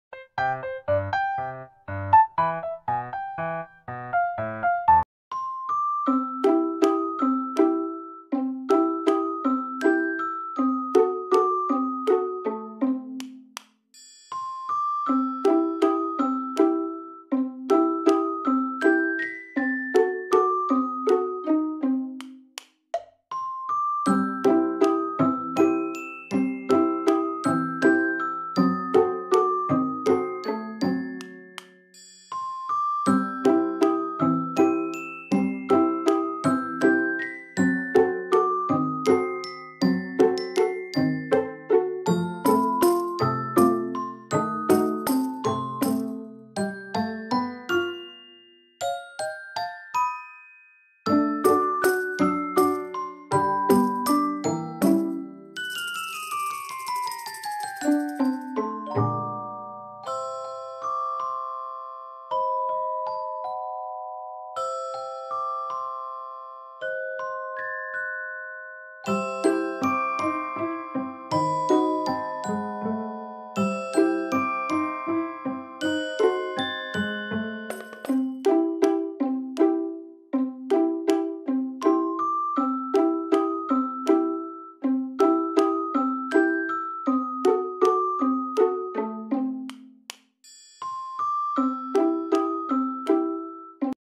Watch my Budgies learning to go inside the cage by themselves ❤️❤️
Watch as my adorable budgies learn to go inside their cage on their own! In this video, we wanted to share the joyful experience.